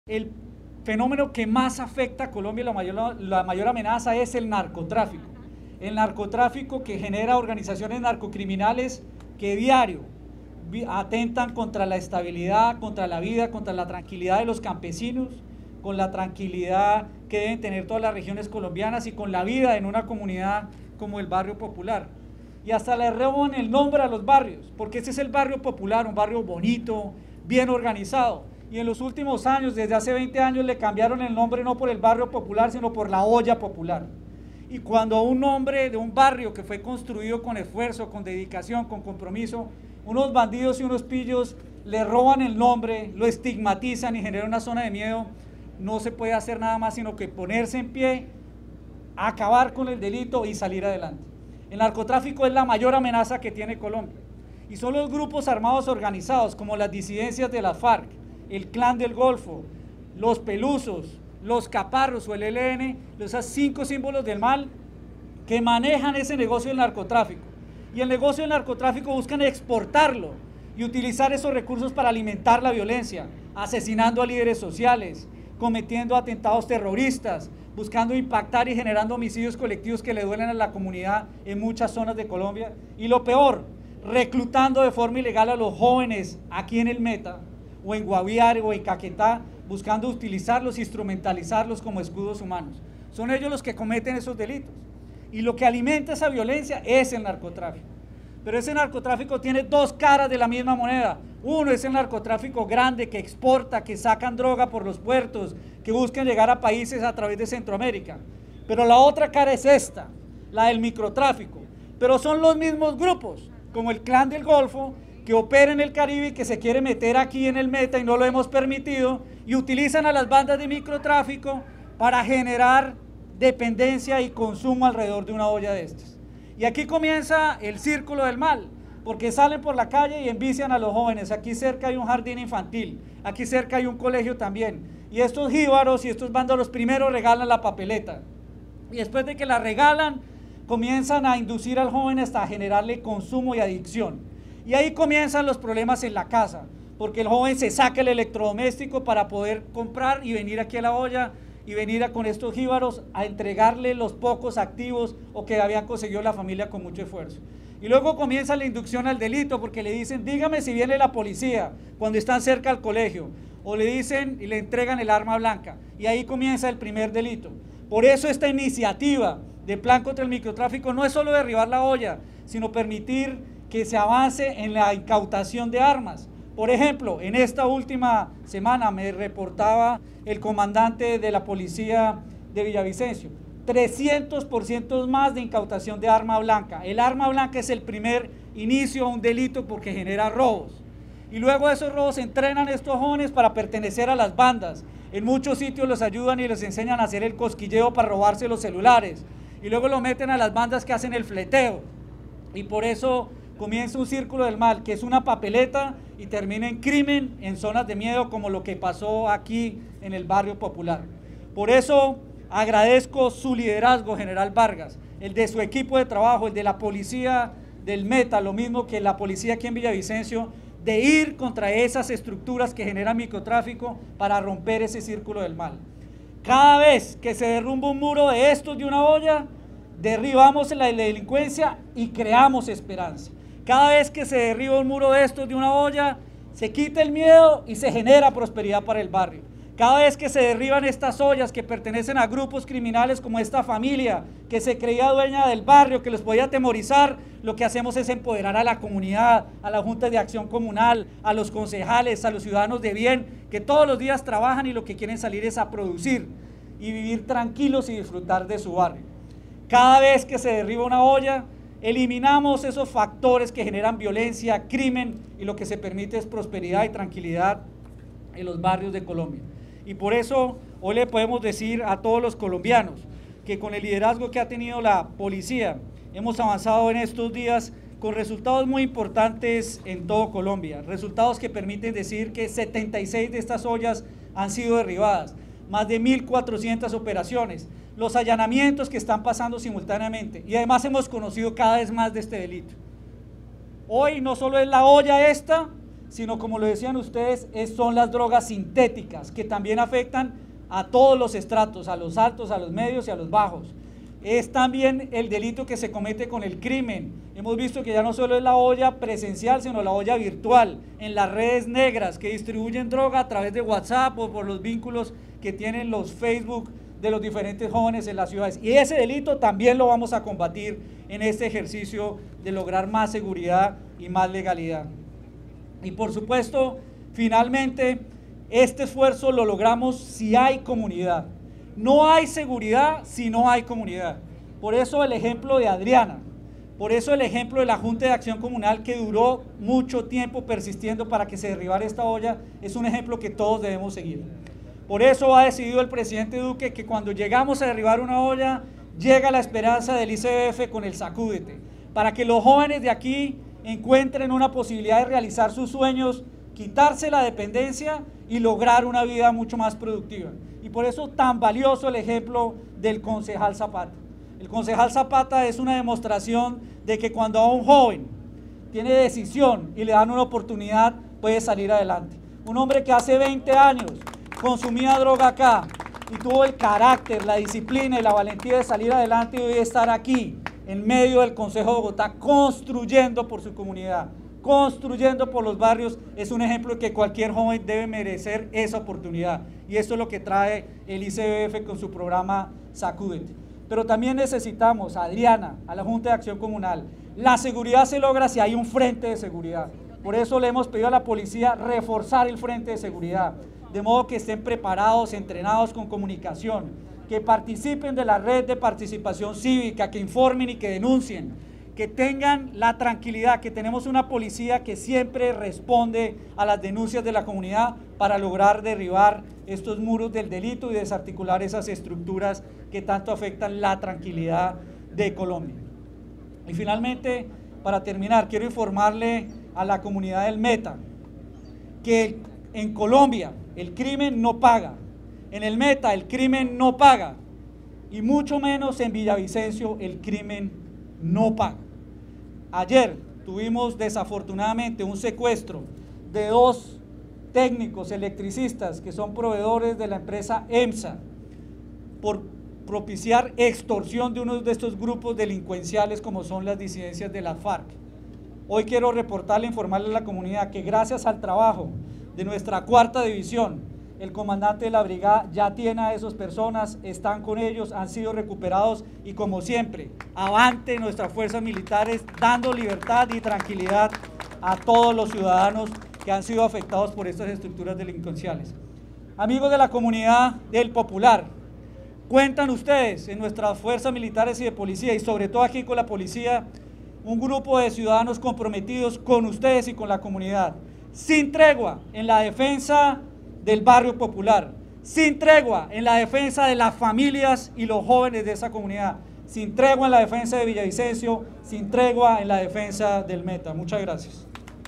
"El mal no seguirá robando la vida de nuestros jóvenes": Mindefensa en demolición de dos 'ollas'
El ministro de Defensa, Diego Molano, lideró la demolición de dos 'ollas' de microtráfico en el barrio Popular de Villavicencio, junto al director de la Policía Nacional, la directora del ICBF, el director de la DIJIN y autoridades locales y regionales del Meta.

"El fenómeno que más afecta a Colombia es el narcotráfico, que genera organizaciones narcocriminales que a diario atentan contra la estabilidad y vida de los ciudadanos. Hasta le roban el nombre a los barrios, por ejemplo, este es el barrio Popular y en los últimos años, le habían cambiado el nombre a 'olla' del Popular", señaló el ministro.

En medio de la demolición, el jefe de la cartera de Defensa, recordó la importancia de los ciudadanos de denunciar para acabar con este tipo de delitos.

"En esta última semana, me reportaban un incremento de 300% más de incautaciones de arma blanca, que es lo que más ha generado robos en la ciudad", explicó Molano.

Asimismo, el Ministro Molano dio un balance de lo ejecutado en estos 38 días del 'Plan 100 Contra el Microtráfico'.

"76 de estas 'ollas' han sido derribadas, más de 1.400 allanamientos en 238 operaciones y 1.839 capturas. Recordando que no hay seguridad, sino hay comunidad y resaltó el ejemplo de los líderes de la junta de acción comunal del barrio Popular con sus constantes denuncias aportes a la comunidad", destacó el ministro.

Finalmente, el Ministro resaltó que gracias al trabajo de las Fuerzas Militares contra los grupos criminales en el Meta se logró el rescate de dos contratistas de la empresa de energía del departamento, que habían sido secuestrados en zona rural de Mesetas, aparentemente, por un cabecilla de extorsión de las disidencias de las FARC ‘Jorge Briceño’.